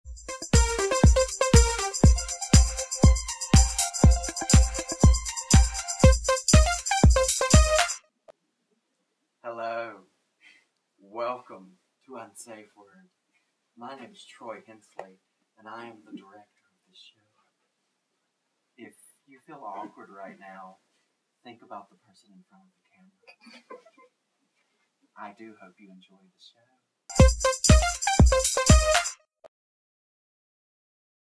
Pillow Talk Episode 3 Meet the Director
Meet the King of Awkward and Director of Unsafe Word, Troy Hencely!

If you don't feel awkward after this video we don't know what to tell you.